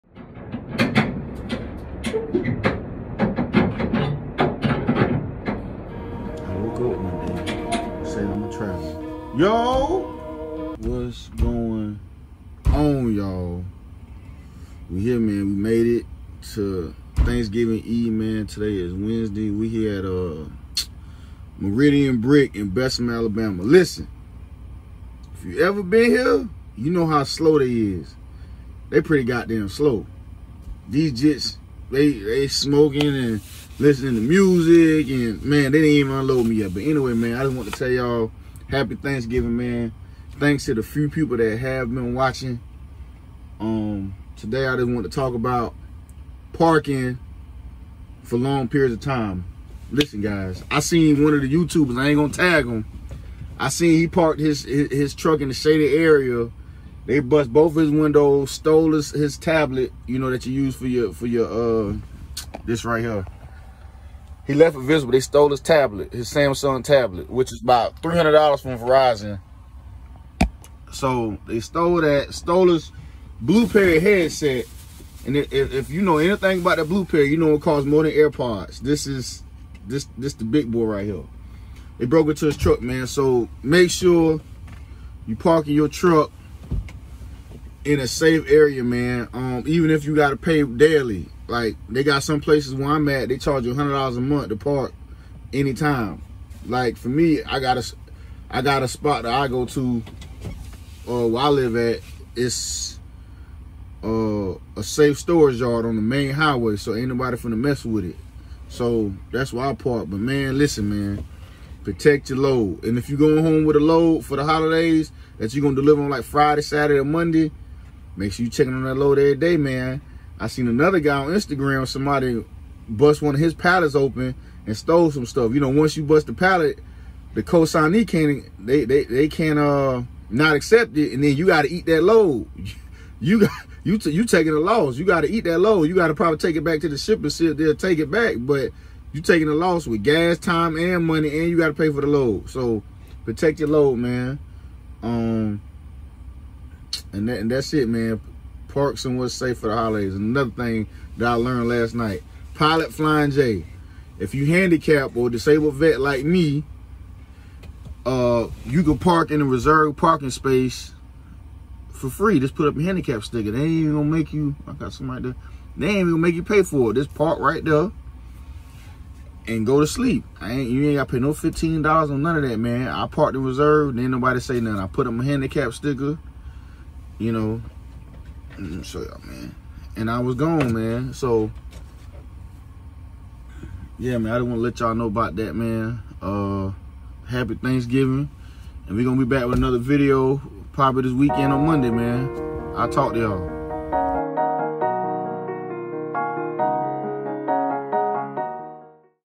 Holiday parking!!
Talking about where you should park SAFELY for the holiday break!!